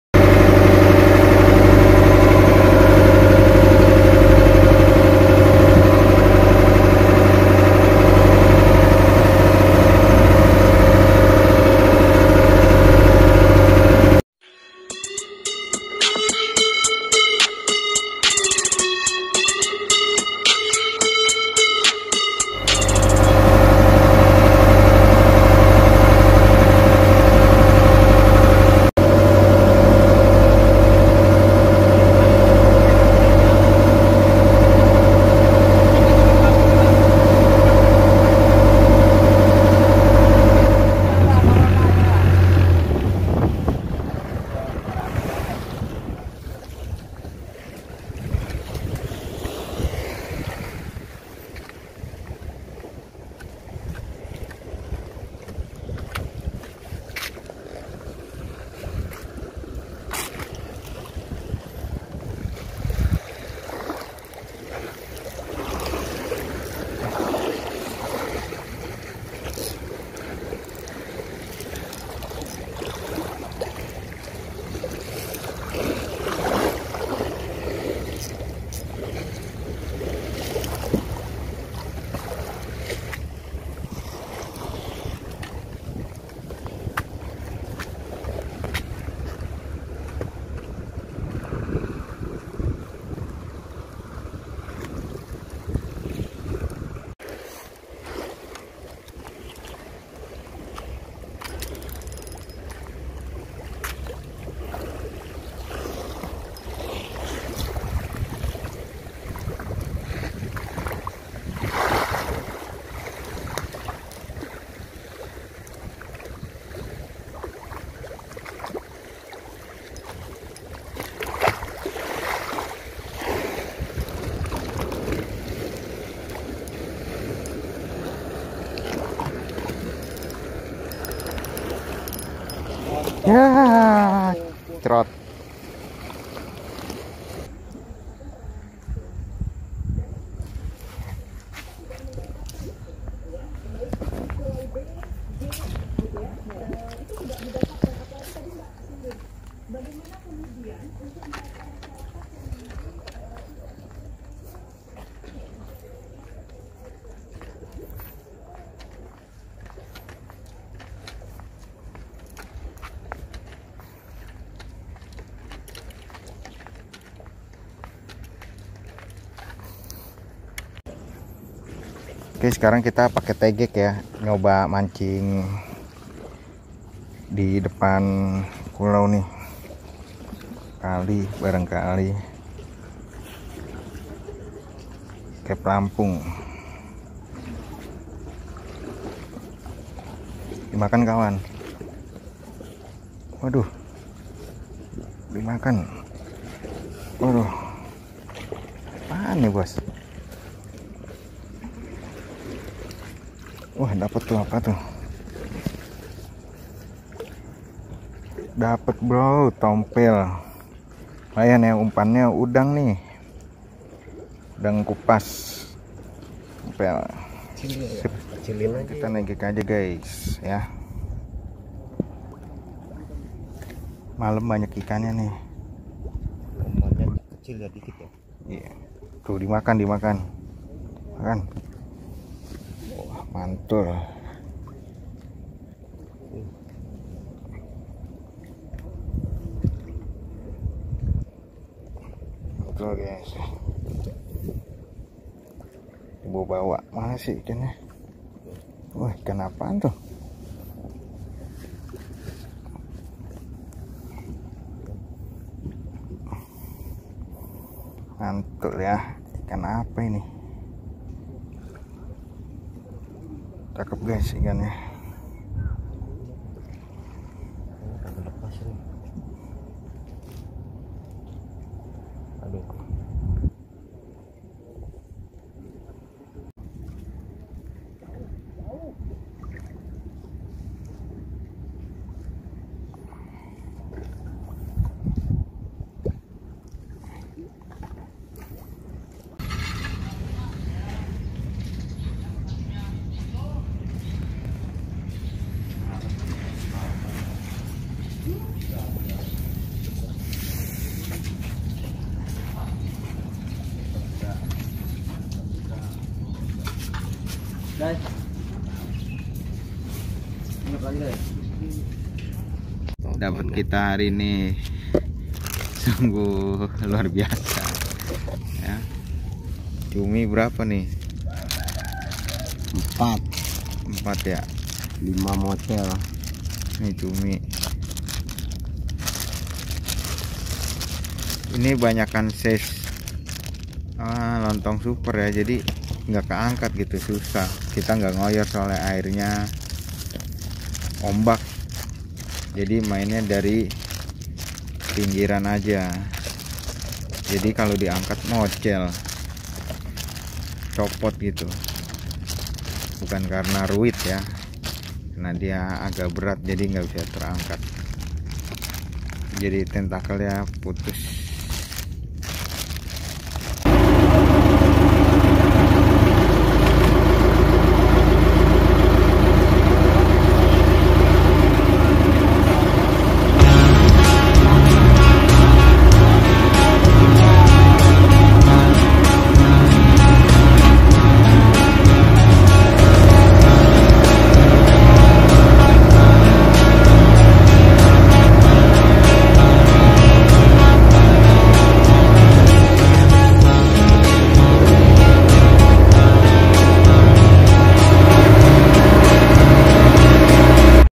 Mancing Malam Di Pulau Onrust, Ngeri Sedap
Suasana saat malam hari di pulau.
Lampu pulau Hanya menyala dari jam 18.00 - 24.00, jangan kaget kalo tiba"lampu padam.

Info kapal Bang Yudi : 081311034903, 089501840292